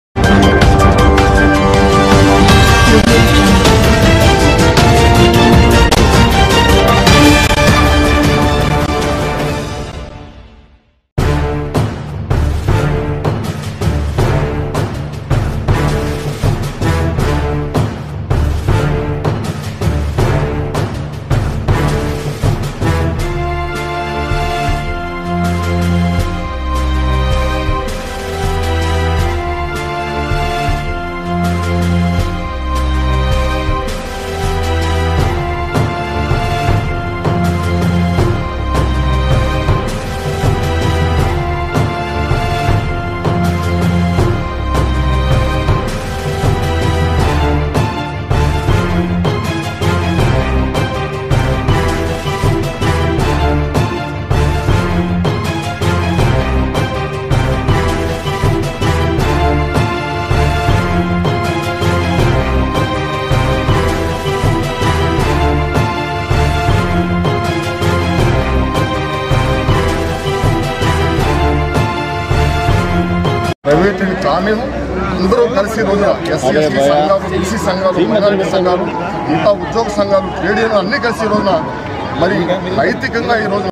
విశాఖ స్టీల్ ప్లాంట్ ప్రైవేటీకరణ ను వ్యతిరేకిస్తూ కారెం శివాజీ ఆధ్వర్యంలో గాజువాకలో పాదయాత్ర
విశాఖ స్టీల్ ప్లాంట్ ప్రైవేటీకరణ ను వ్యతిరేకిస్తూ AP SC,ST కమీషన్ మాజీ చైర్మెన్ కారెం శివాజీ ఆధ్వర్యంలో గాజువాకలో పాదయాత్ర